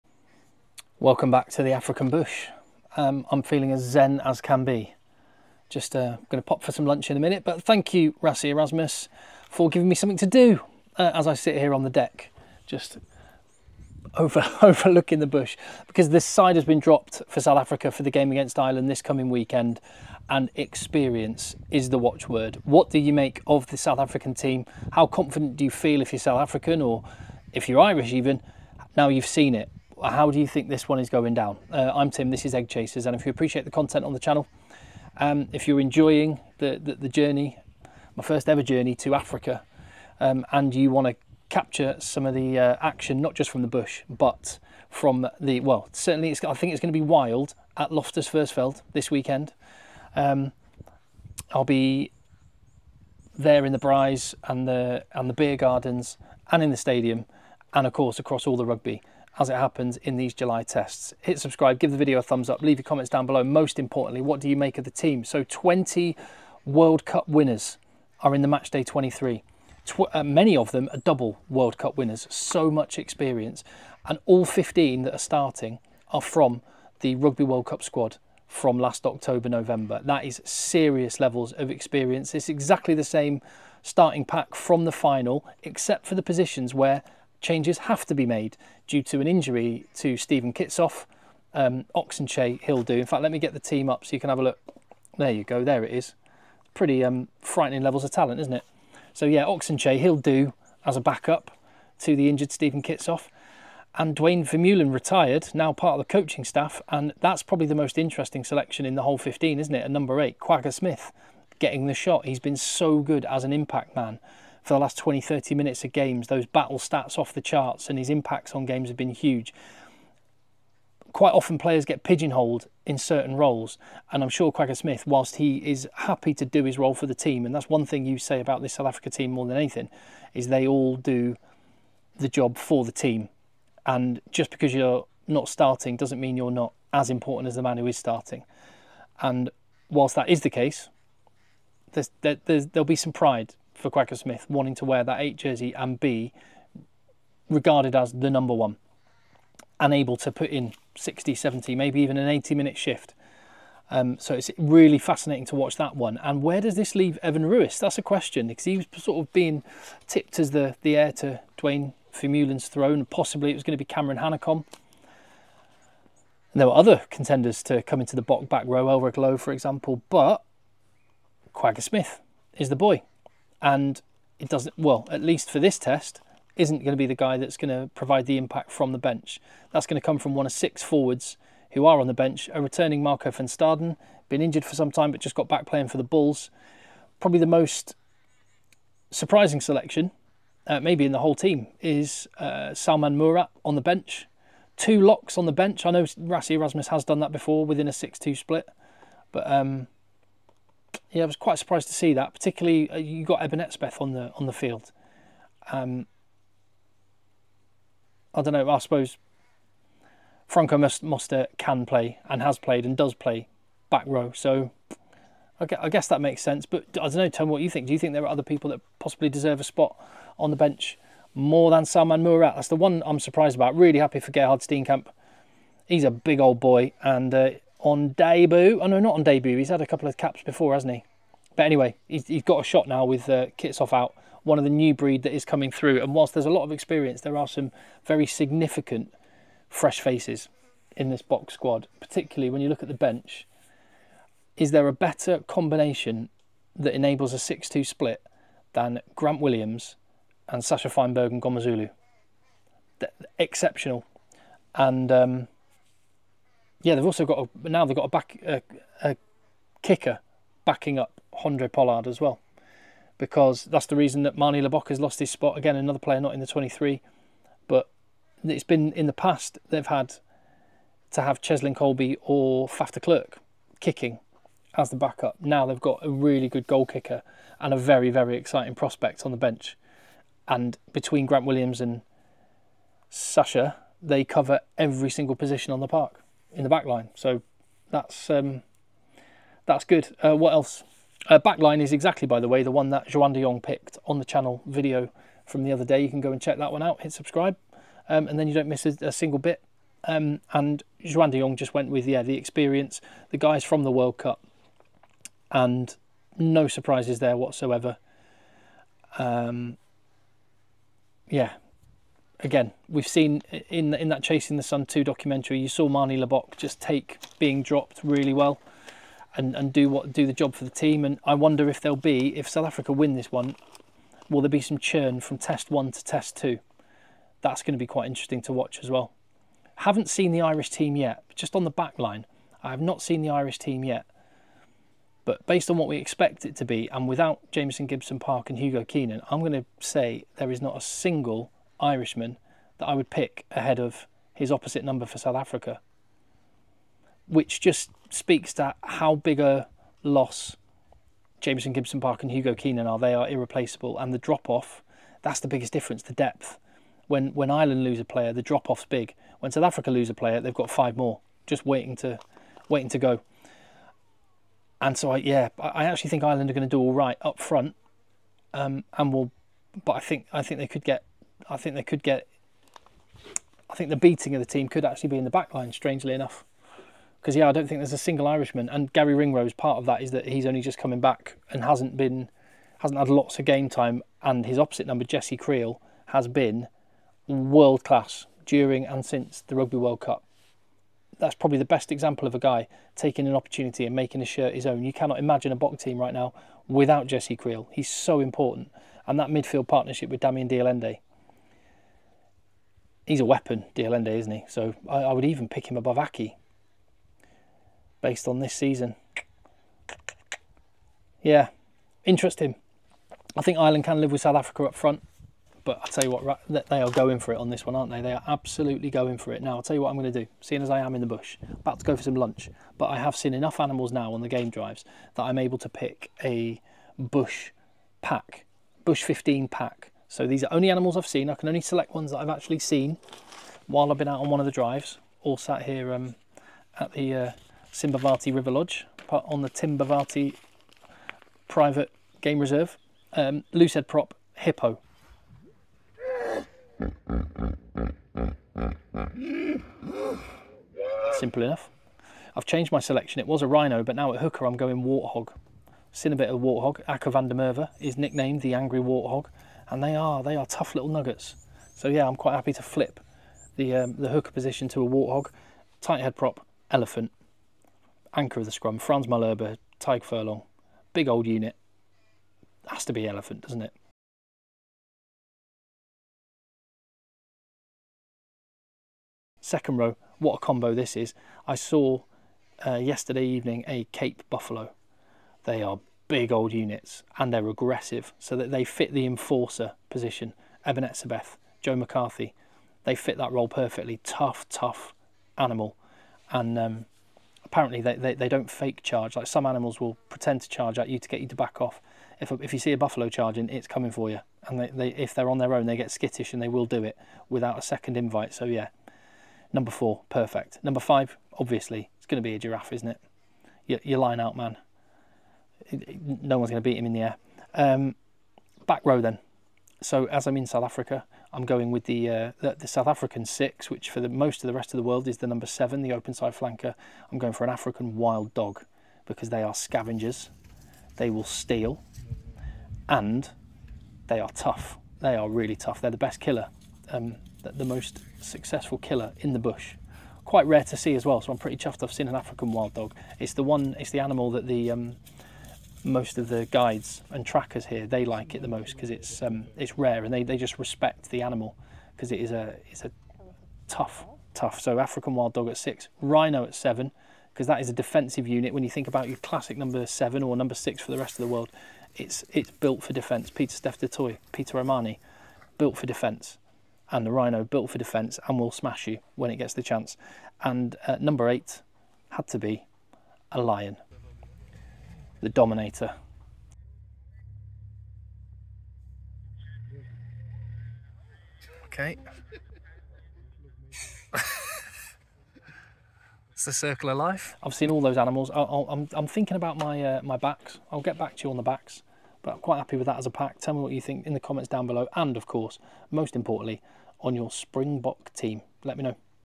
KWAGGA TIME! | SOUTH AFRICA TEAM | SELECTION REACTION
A seriously experienced team will take to the field at Loftus Versfeld on Saturday against Ireland. My thoughts on selection.
PLUS I think i've nailed down my pack for the Bush XV, a challenge I've set myself whilst staying at Simbavati River Lodge in the Timbavati Private Game Reserve